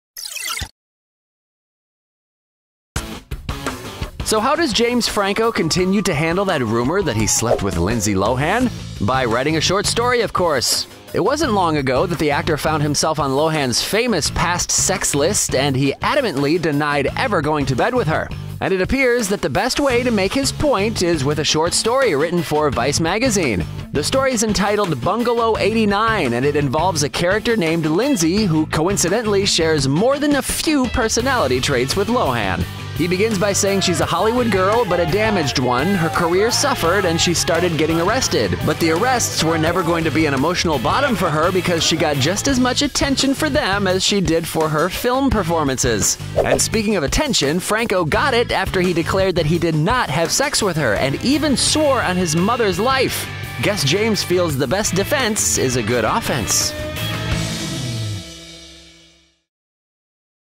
James Franco Uses Short Story to Deny Sleeping with Lohan | Splash News TV
James Franco used his writing skills to pen a short story called Bungalow 89, which appears to be an eloquently worded denial that he ever slept with Lindsay Lohan.